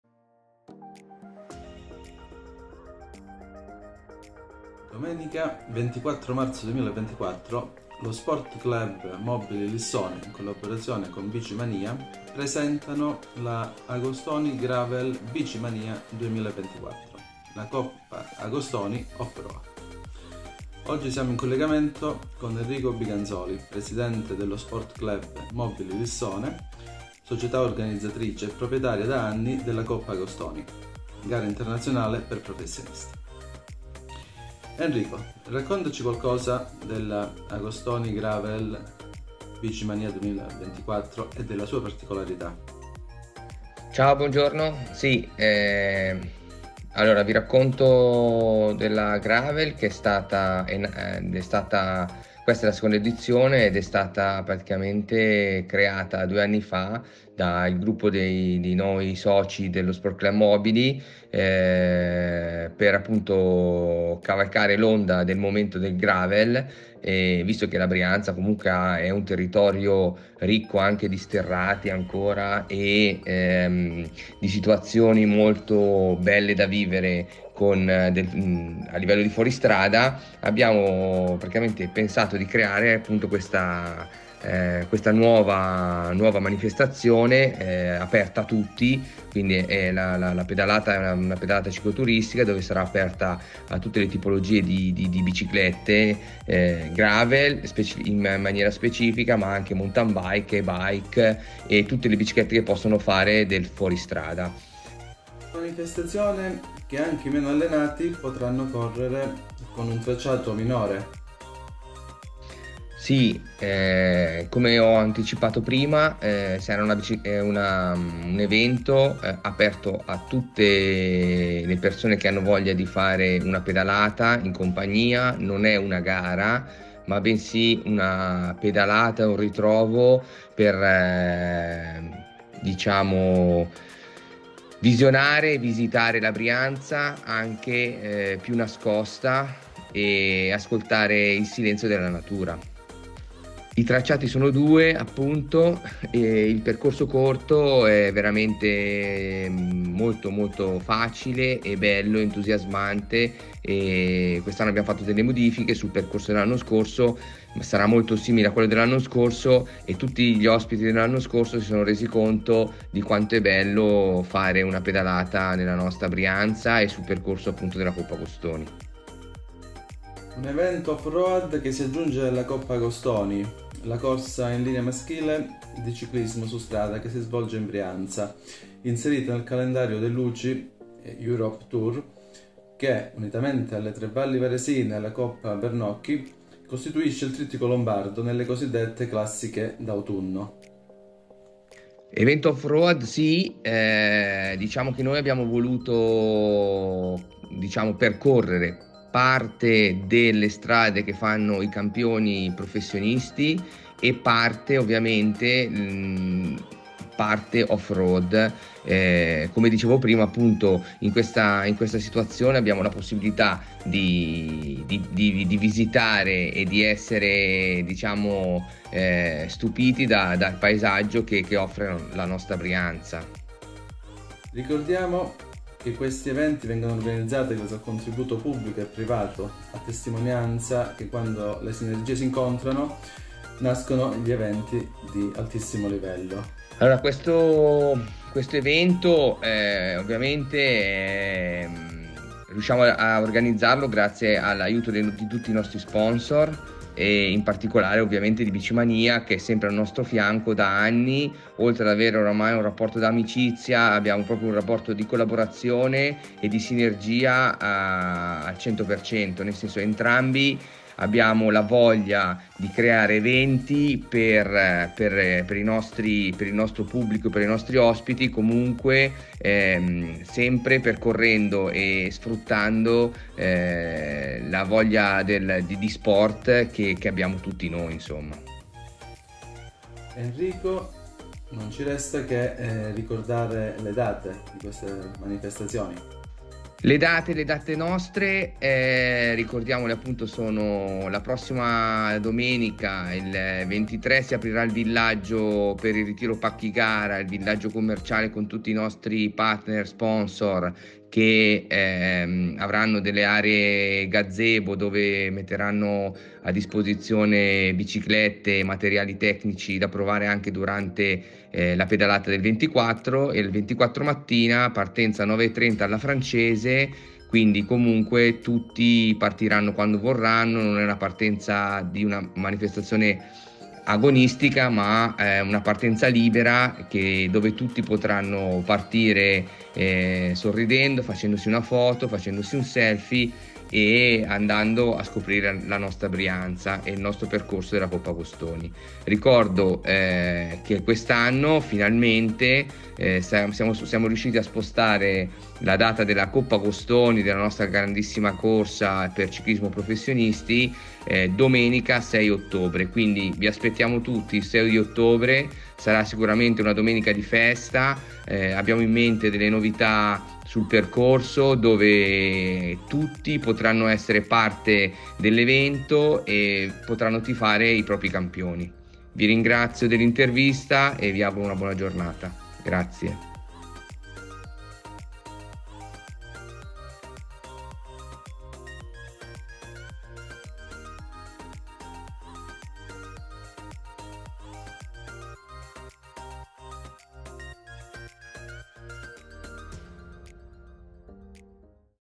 Enrico Biganzoli, Presidente dello Sport Club Mobili Lissone
Intervista a Enrico Biganzoli, Presidente dello Sport Club Mobili Lissone - 24 marzo 2024 - Sport Club Mobili Lissone, in collaborazione con Bicimania, presentano “AGOSTONI GRAVEL BICIMANIA 2024”, la Coppa Agostoni off road - A cura di Antonio Abbruzzese e Tania Klim